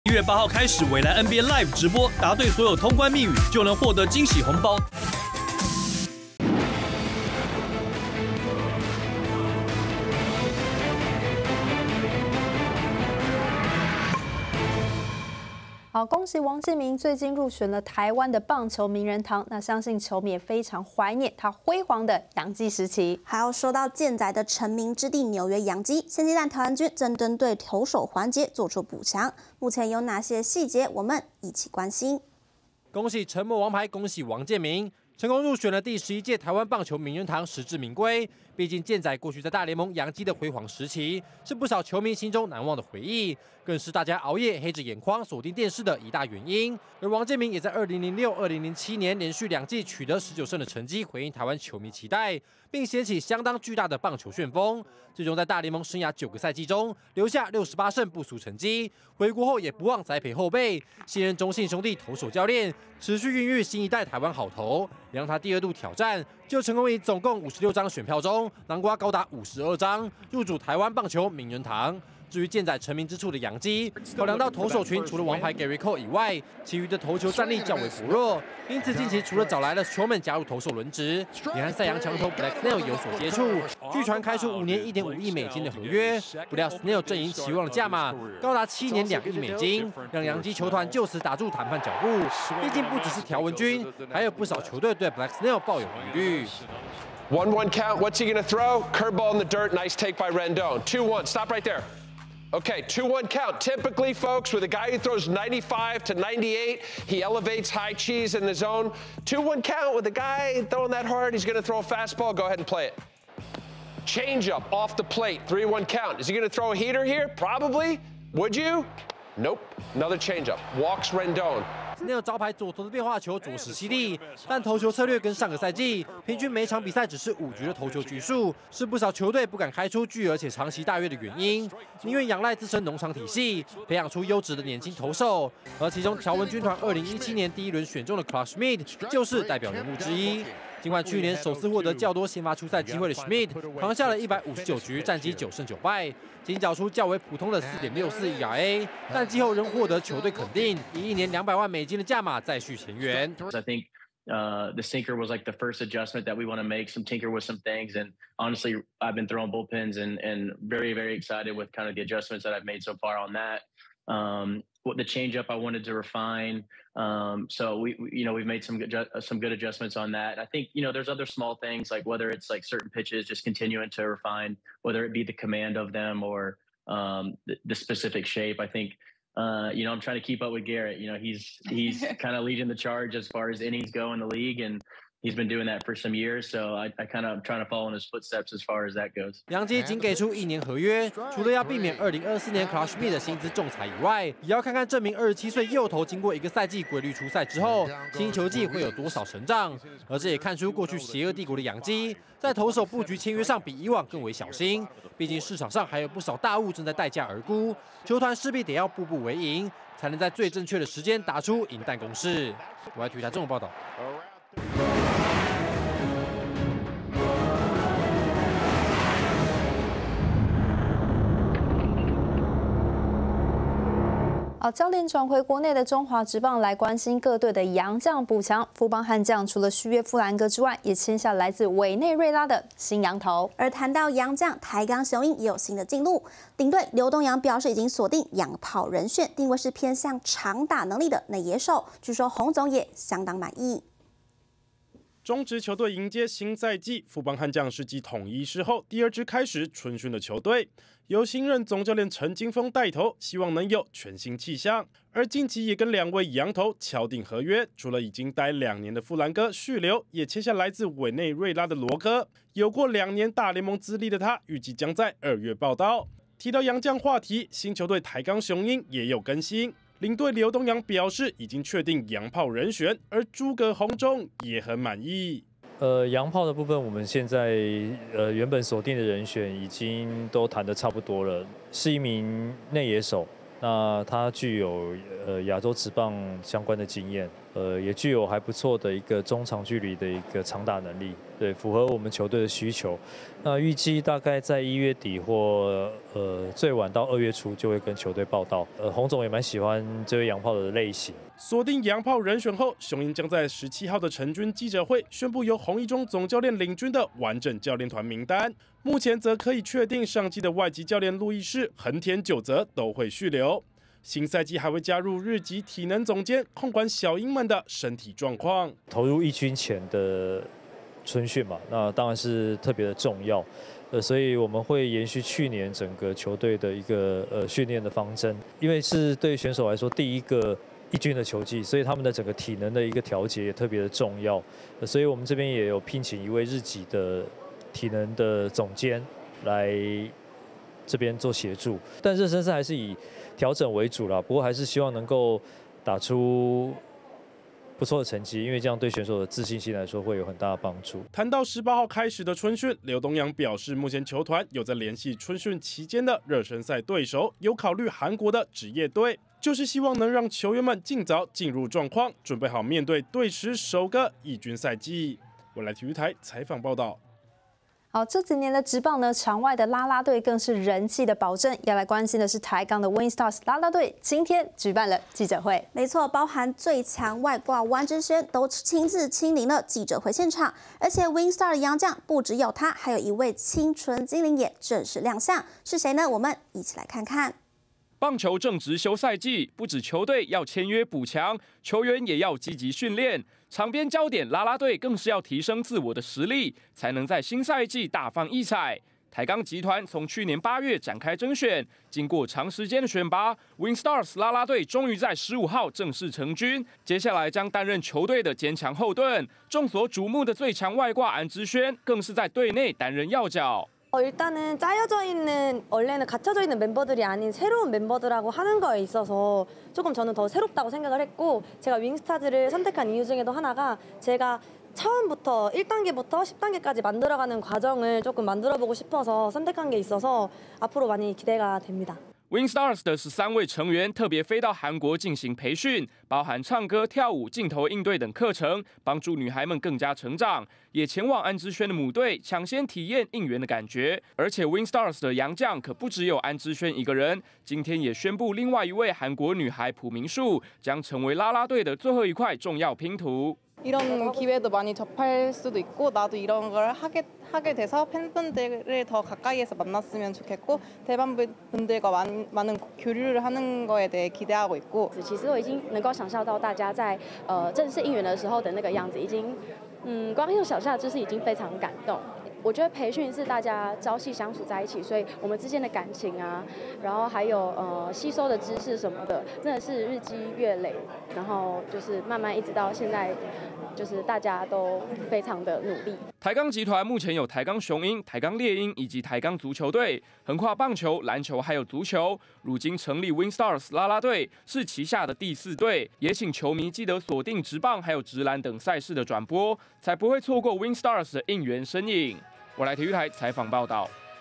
1/15 王建民入選台灣棒球名人堂 雄鷹聚焦最強外援安芝儇
00:00 沉默王牌王建民 入選台灣棒球名人堂
03:35 台鋼瞄準洋砲人選 悍將簽下第二位洋投
06:08 雄鷹全面備戰 聚焦最強外援安芝儇